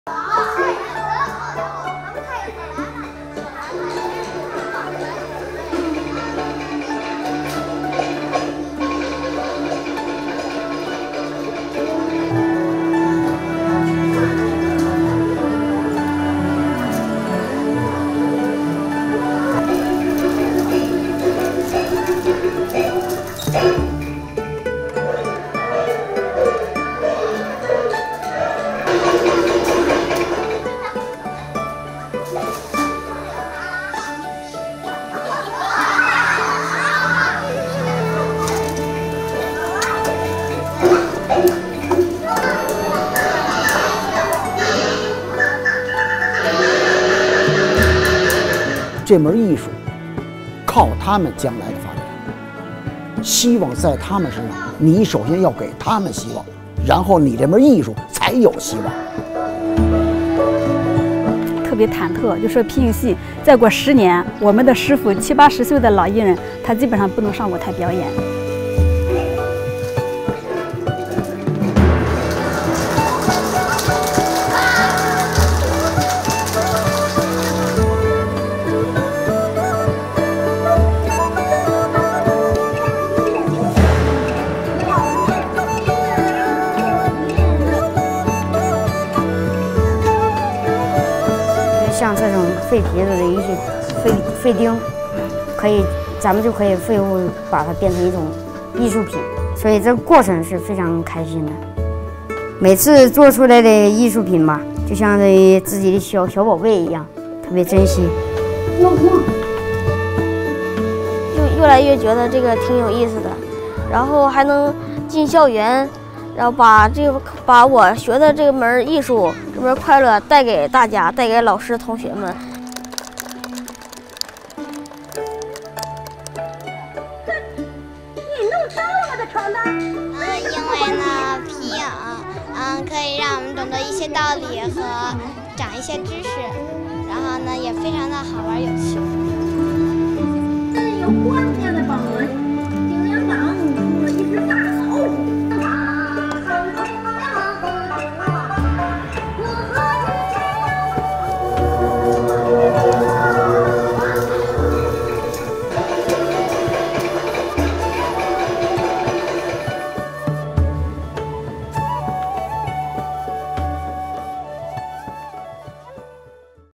Ancient Chinese art of shadow puppet theatre struggling to survive in the modern era
A popular form of entertainment for more than 2,000 years, Chinese shadow puppetry is now an endangered craft. For the past few decades, traditional shadow theatres have been struggling to attract audiences and the master puppeteers of the old generation are worried the art form will die with them.